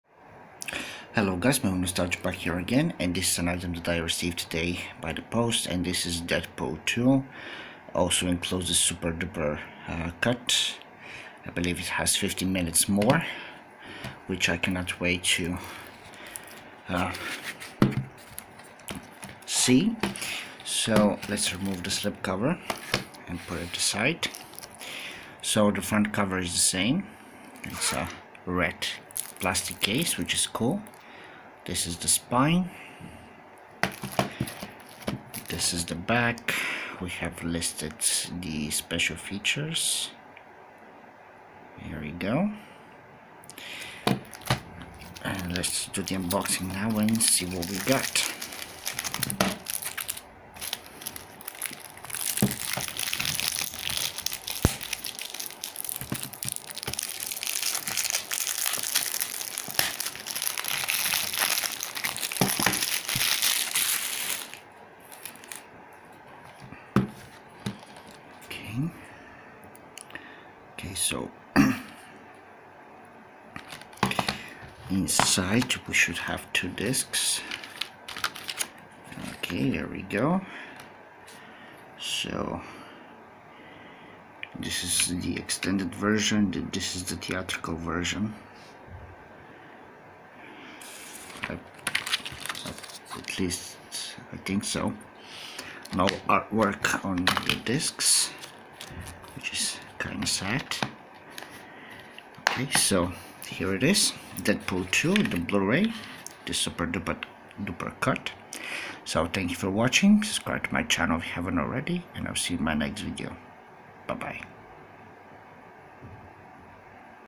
Deadpool 2 Blu-ray Unboxing
It includes the super duper cut with additional 15 minutes of footage . Awesome release !